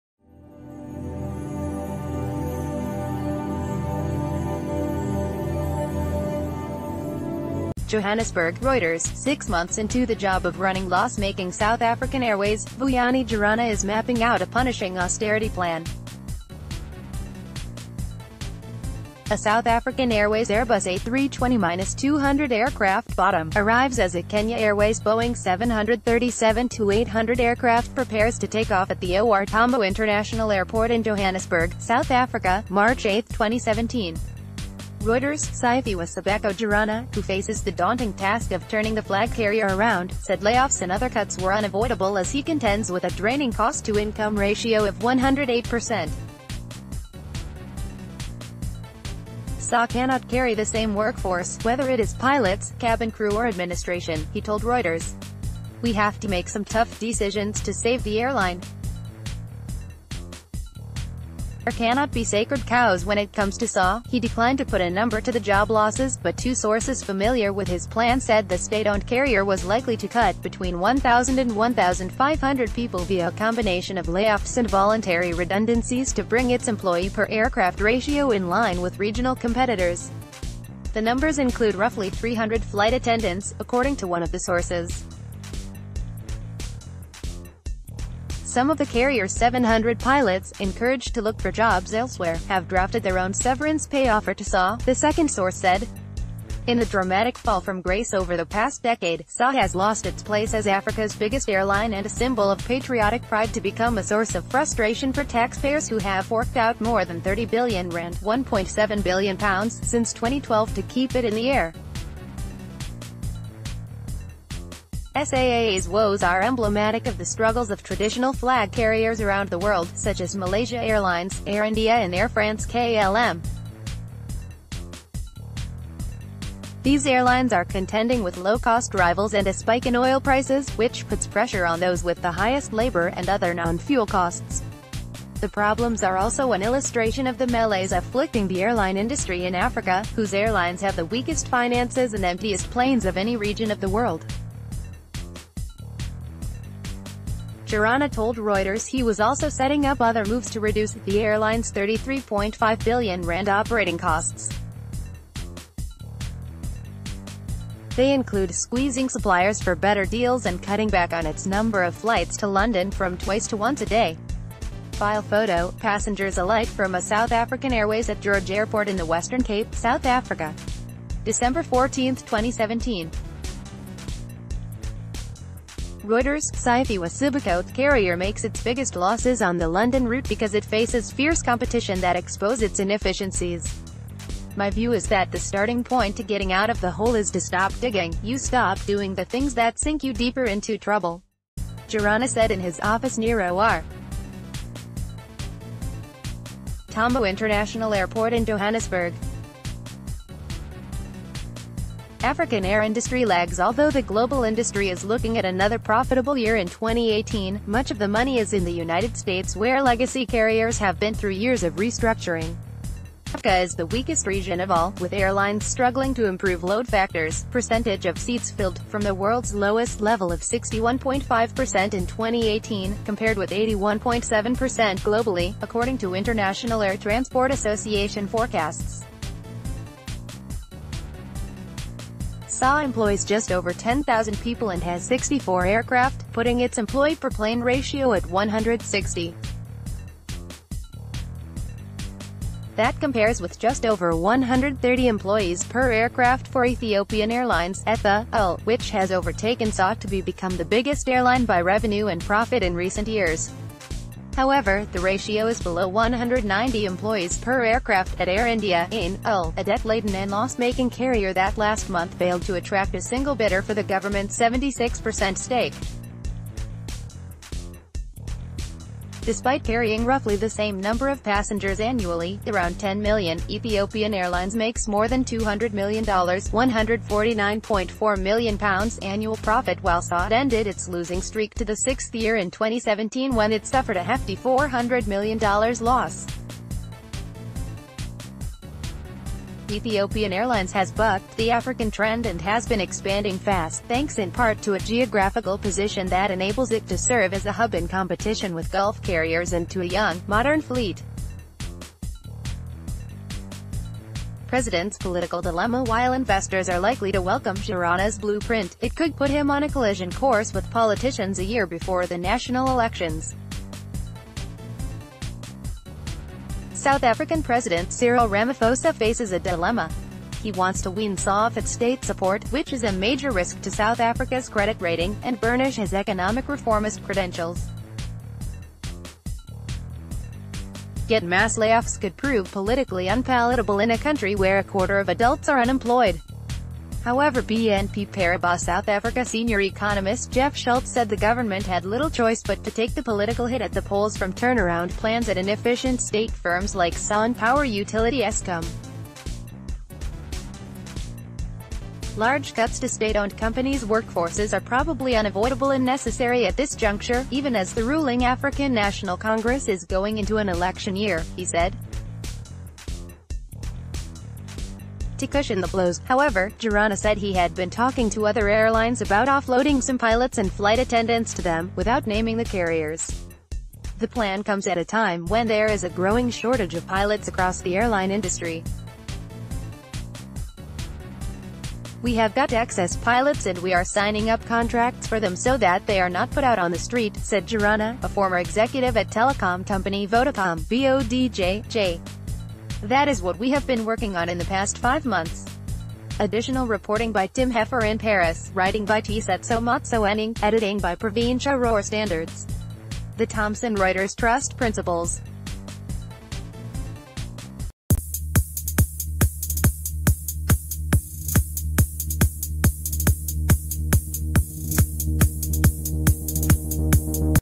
Breaking News | 'No sacred cows' - South African Airways boss plans deep cuts
JOHANNESBURG (Reuters) - Six months into the job of running loss-making South African Airways, Vuyani Jarana is mapping out a punishing austerity plan. A South African Airways Airbus A320-200 aircraft (bottom) arrives as a Kenya Airways Boeing 737-800 aircraft prepares to take off at the OR Tambo International Airport in Johannesburg, South Africa, March 8, 2017. REUTERS/Siphiwe Sibeko Jarana, who faces the daunting task of turning the flag carrier around, said layoffs and other cuts were unavoi...